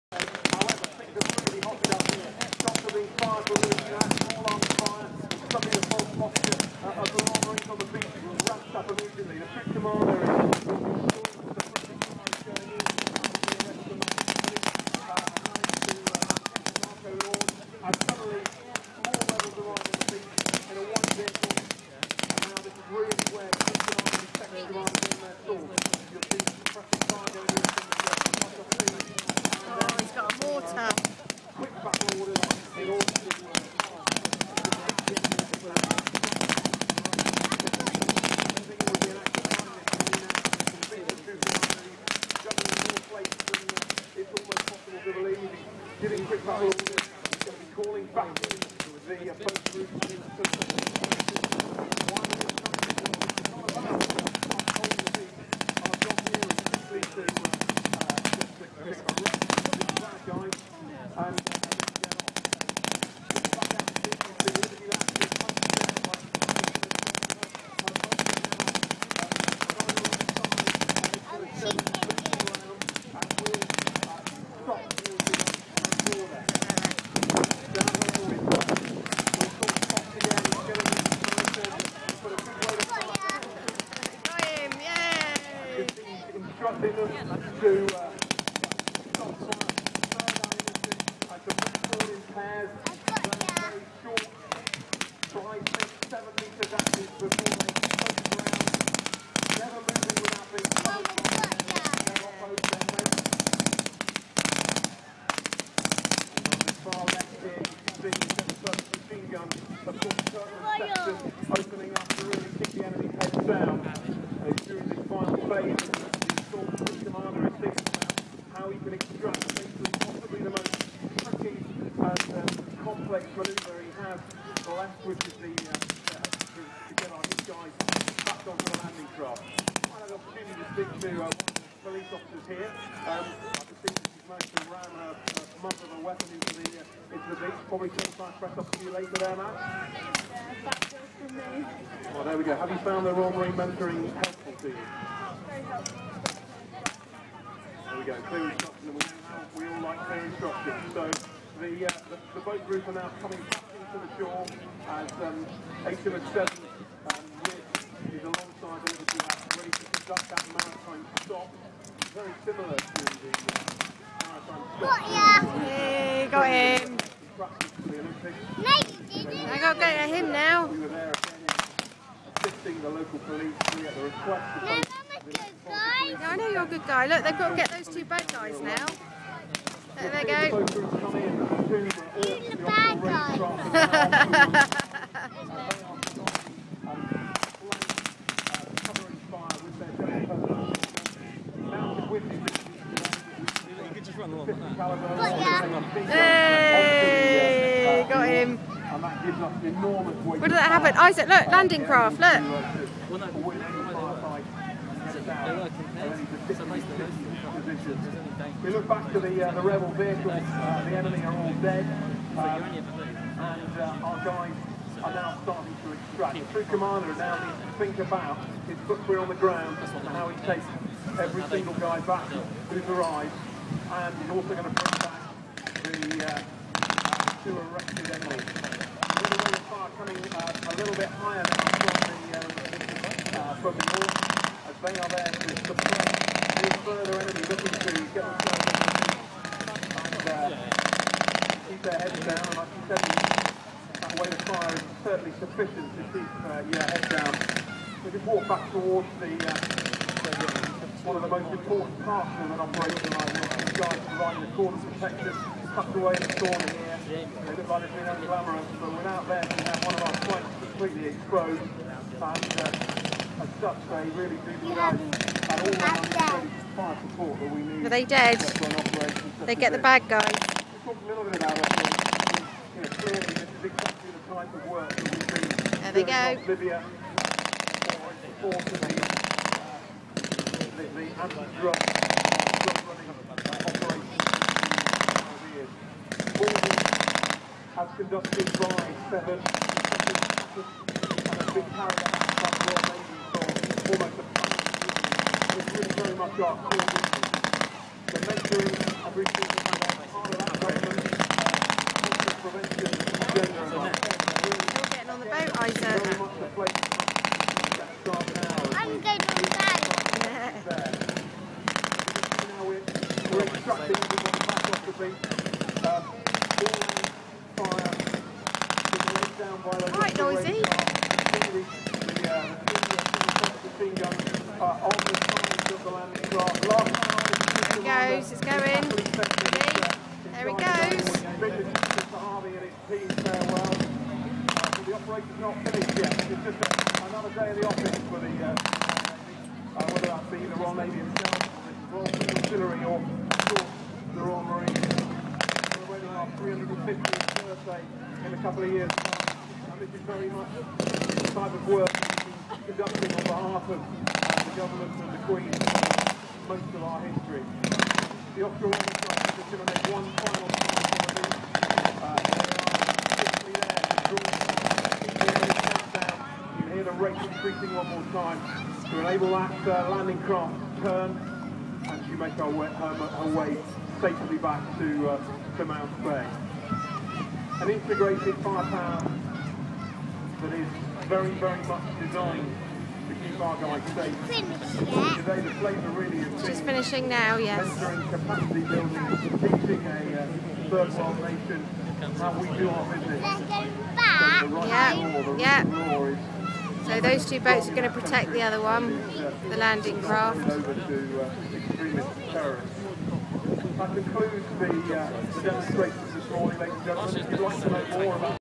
Beach landing - Bournemouth Airfest 2012
The beach landing from Bournemouth airfest 2012 featuring the marines!